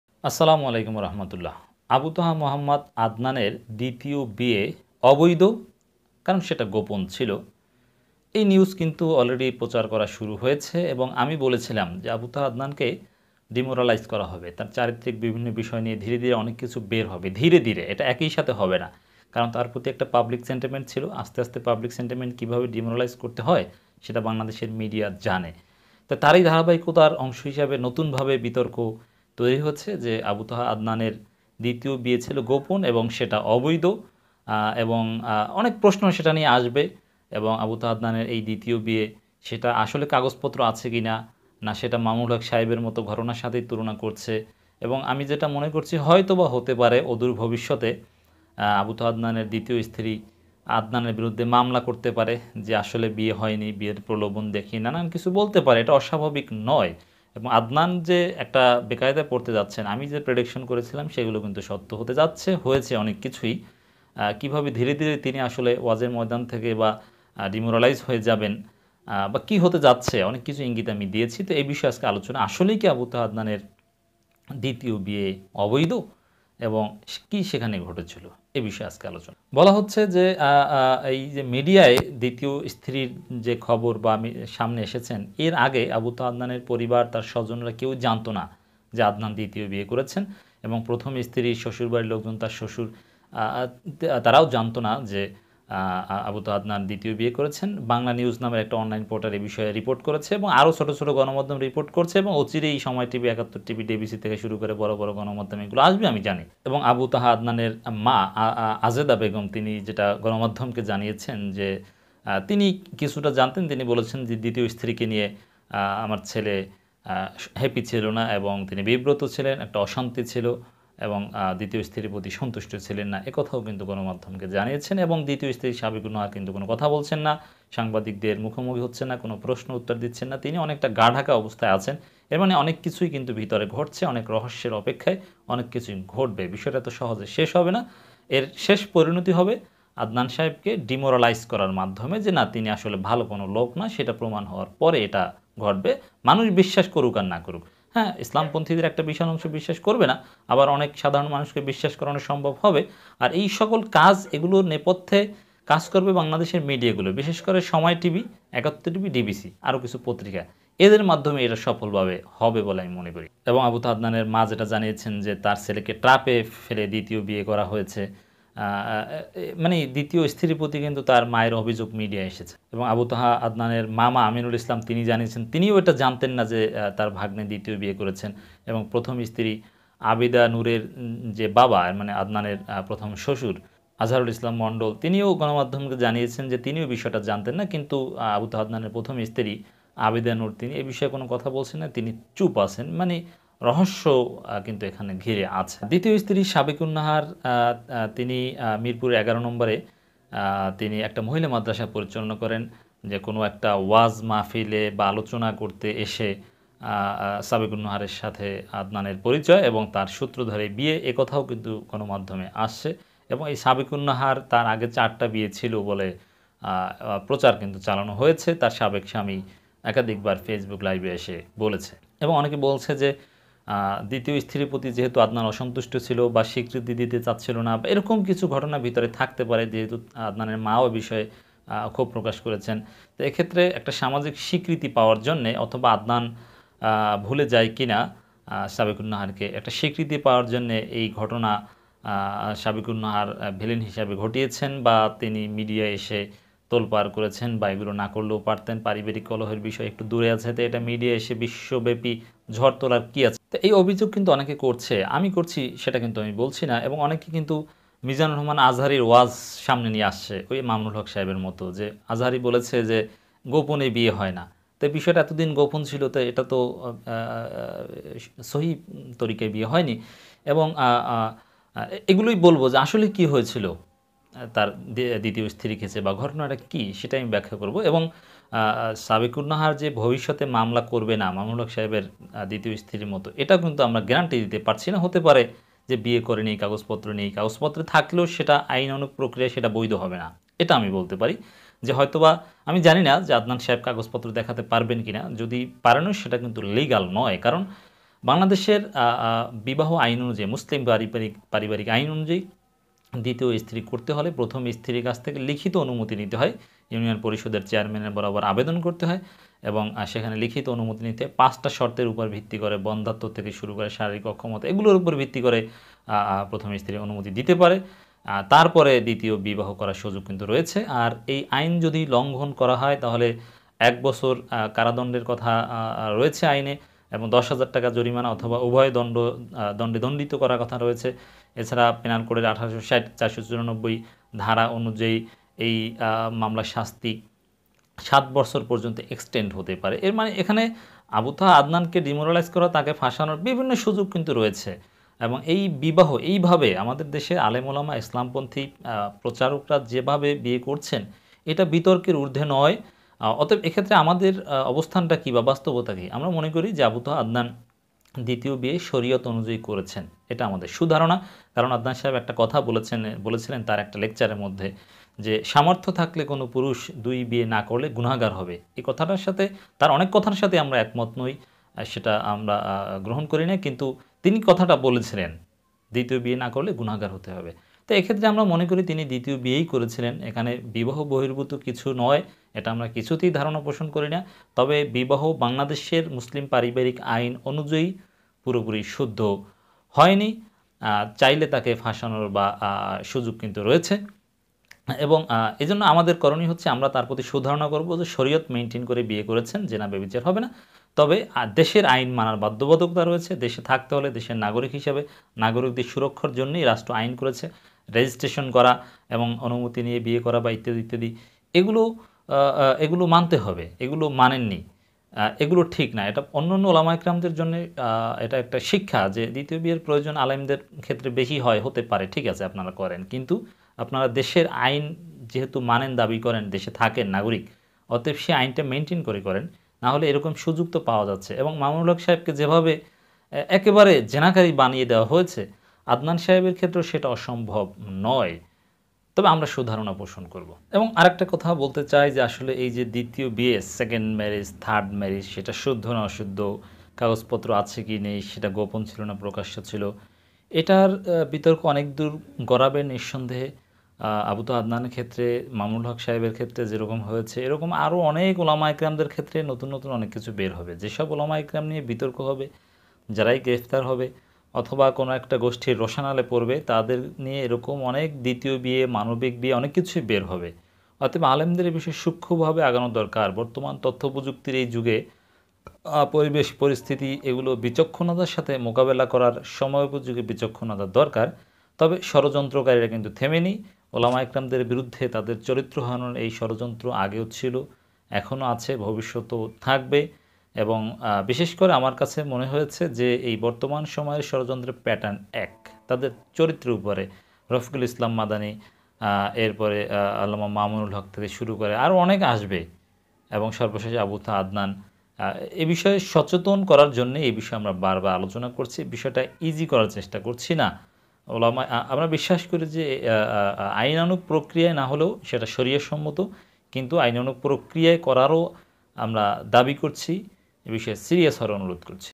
আবু ত্বহা আদনানের দ্বিতীয় বিয়ে অবৈধ||মামলা করতে পারেন দ্বিতীয় স্ত্রী সাবেকুন্নাহার
এতেকাফে নয় আবু ত্বহা আদনান ভারতে হজ্জে গিয়েছিলেন||নেপথ্যে জামাত শিবির||রমযানের বাইরে কী এতেকাফ হয়?||
আর কত নাটক আদনানকে নিয়ে||